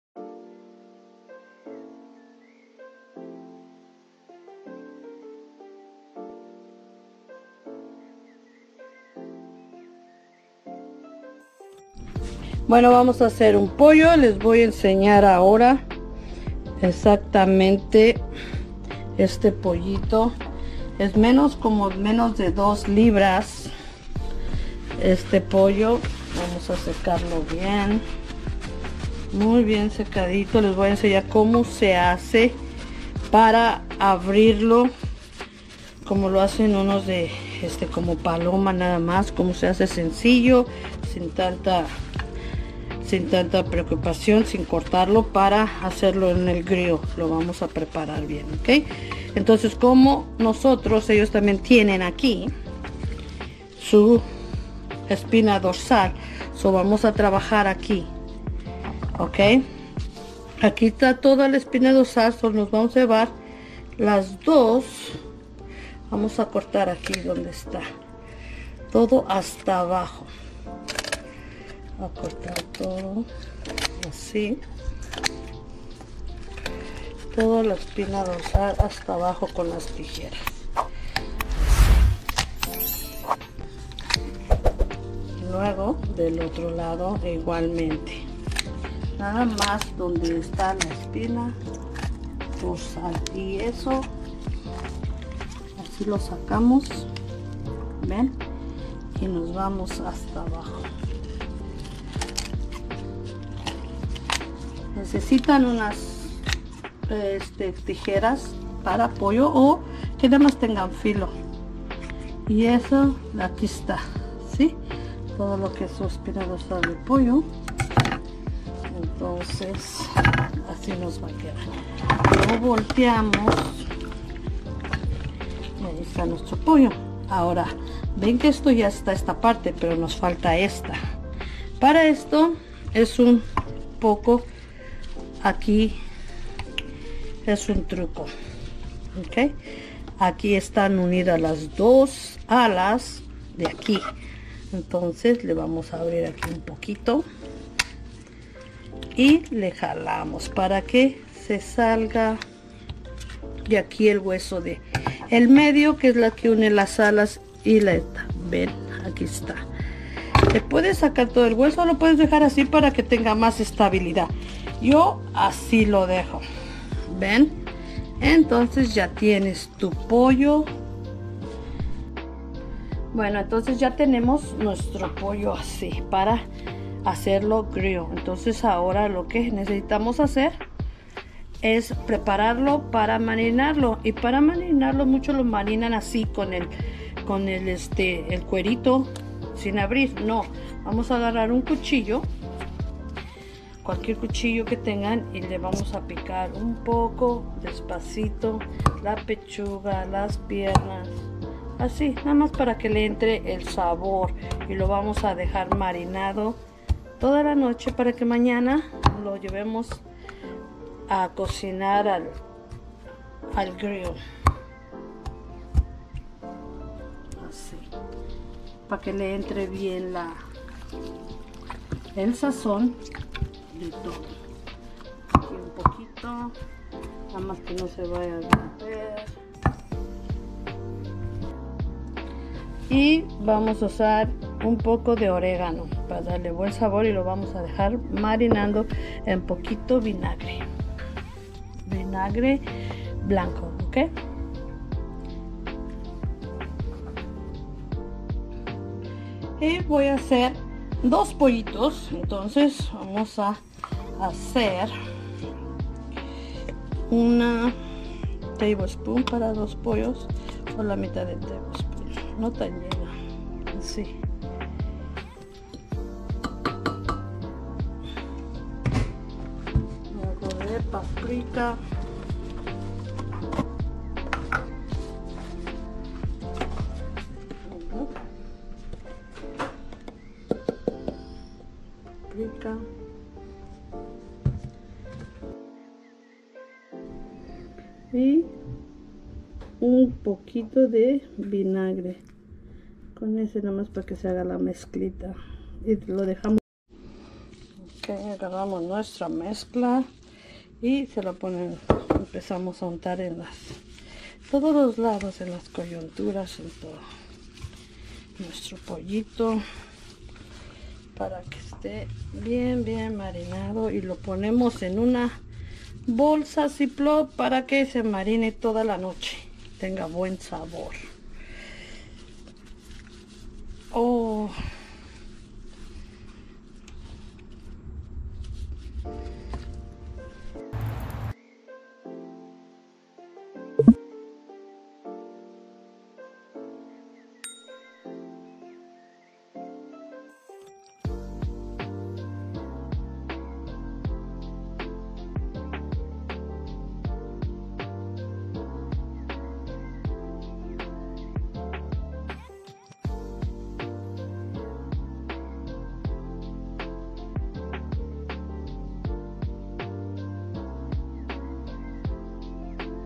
La Forma Mas Facil de Cortar El Pollo Estilo Mariposa
¿Qué es el pollo mariposa?
El pollo mariposa es la forma perfecta de cocinar y servir un pollo entero y, con esta sencilla guía, podrás hacerlo tú mismo en 2 sencillos pasos. 'Mariposa' (o 'Spatchcock') es un término utilizado para describir un pollo al que se le ha quitado la columna vertebral para poder aplanarlo .